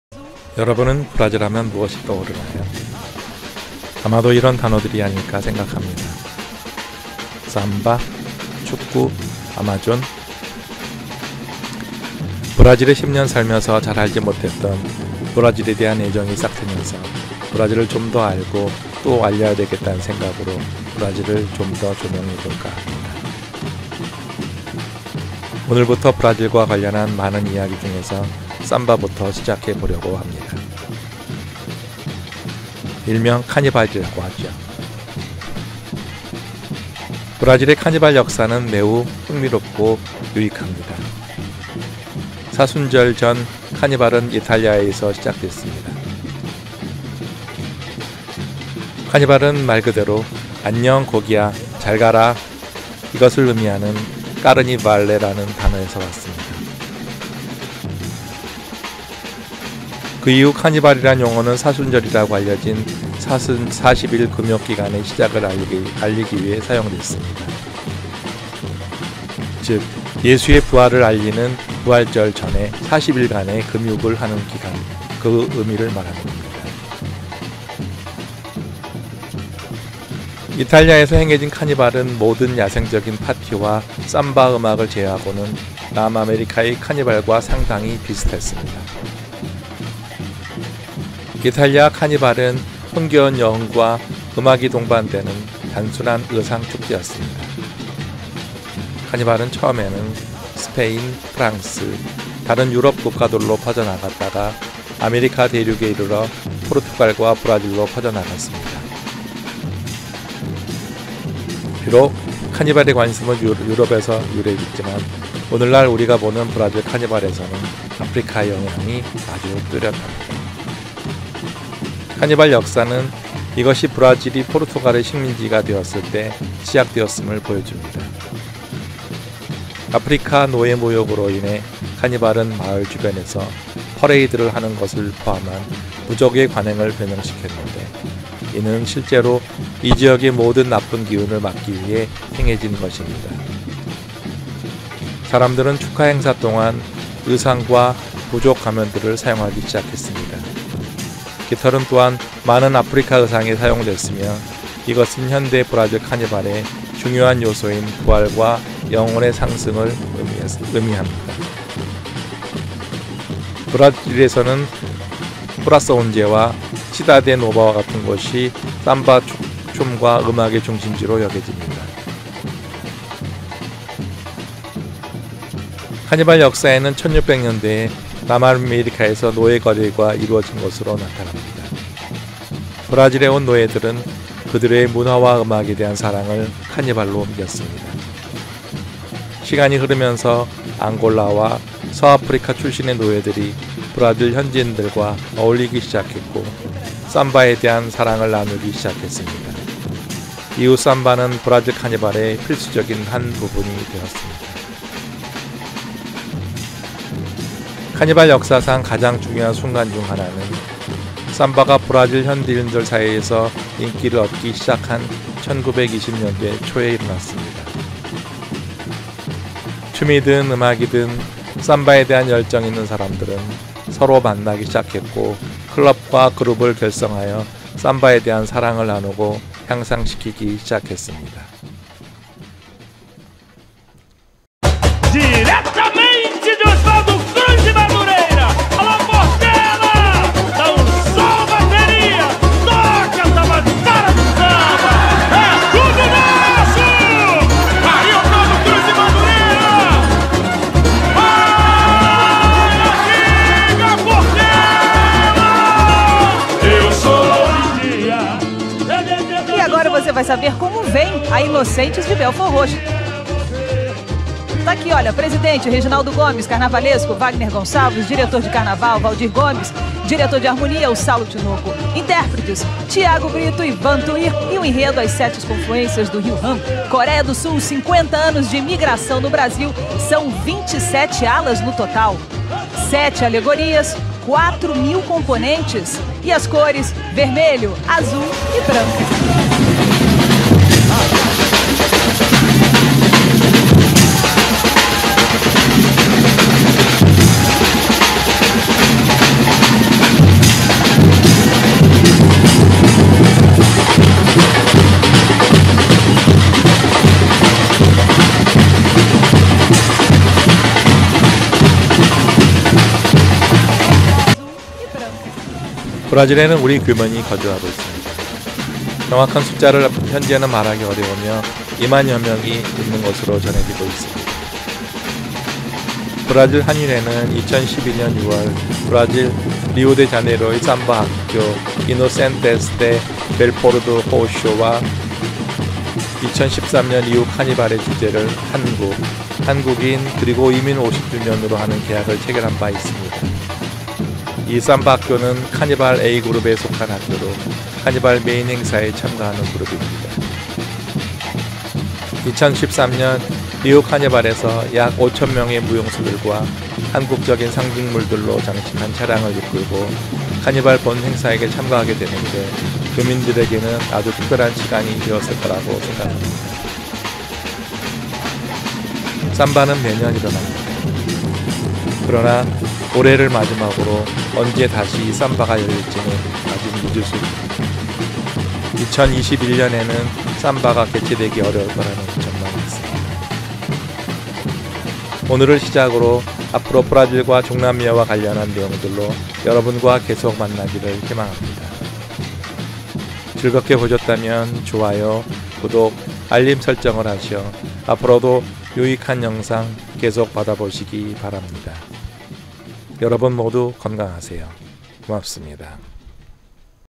[브라질 문화1]쌈바의 원조가 따로 있다는데, 윈조논란이 벌어진다?
삼바는 어디서 시작됐을까요?
여러분은 브라질’하면 무엇이 떠오르나요? 아마도 이런 단어
들이 아닐까 생각합니다. 삼바, 축구, 아마존!

삼바의 배경을 알아 봅니다.

코로나 시대의 어려움을 조금이나마 푸는 시간 갖기를 바랍니다.